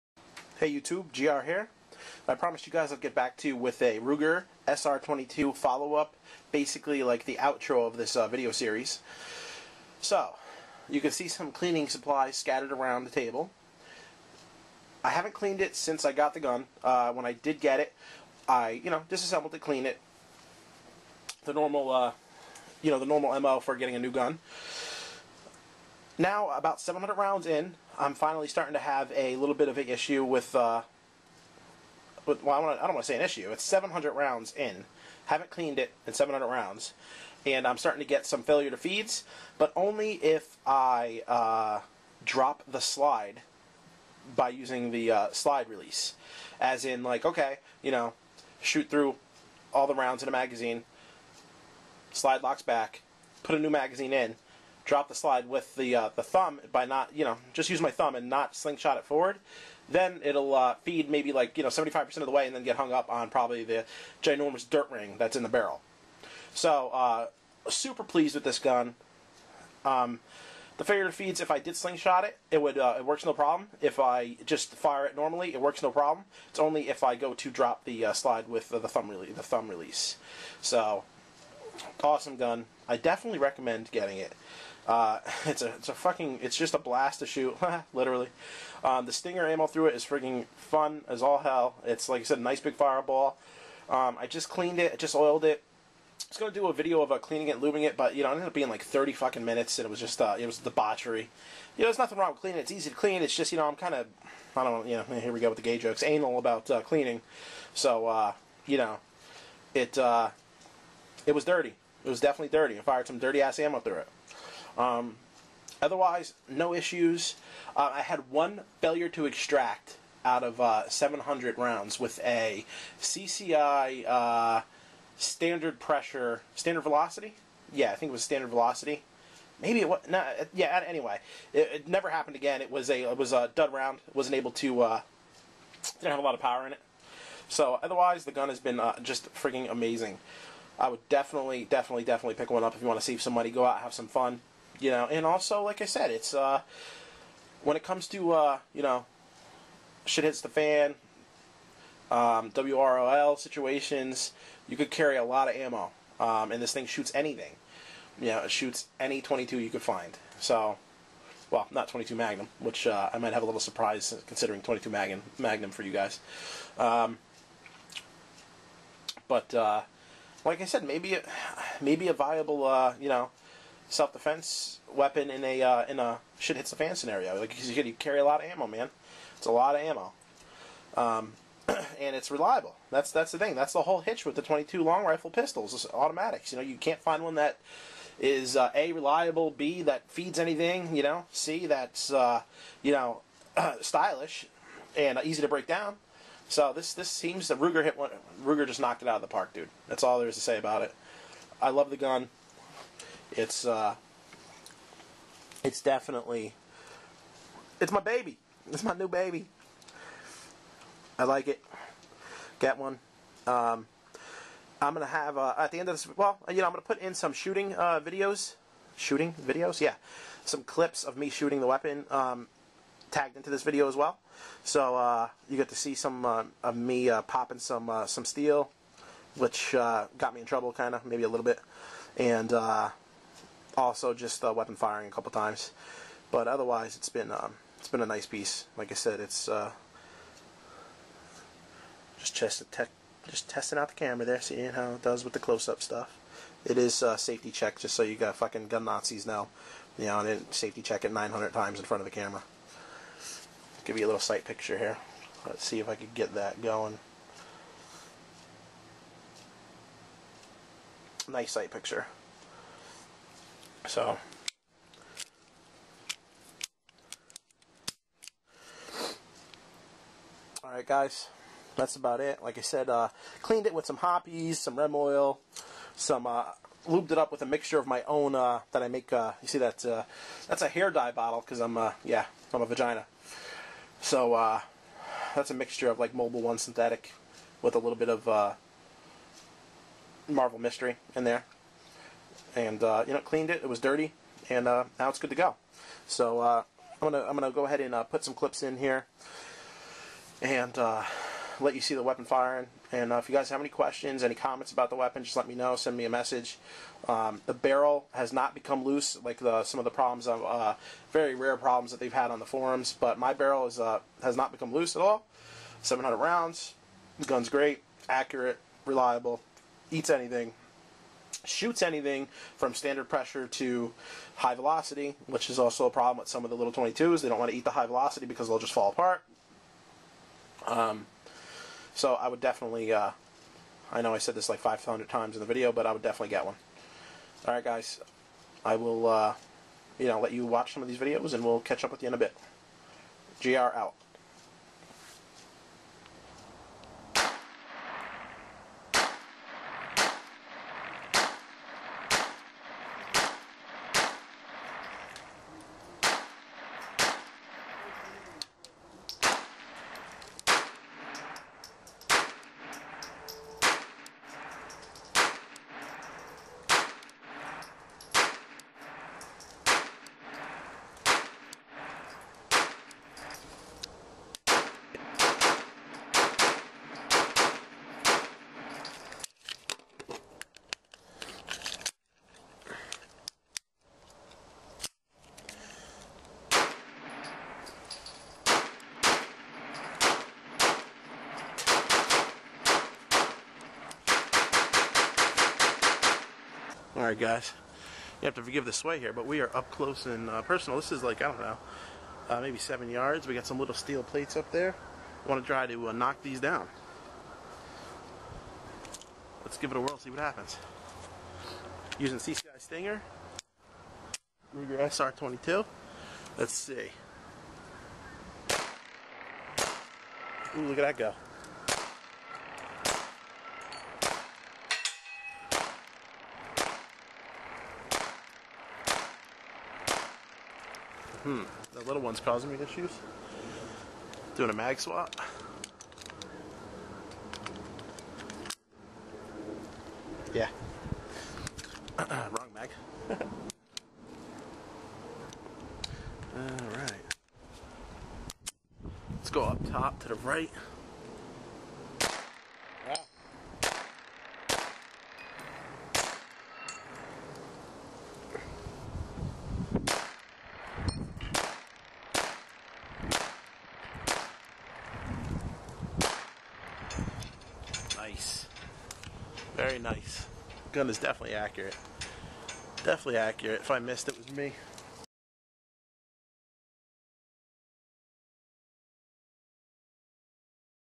Ruger SR-22 pistol wrap up and action shots...
Great gun! Wrapping up the ruger sr-22 series of reviews. Some shooting videos at the end.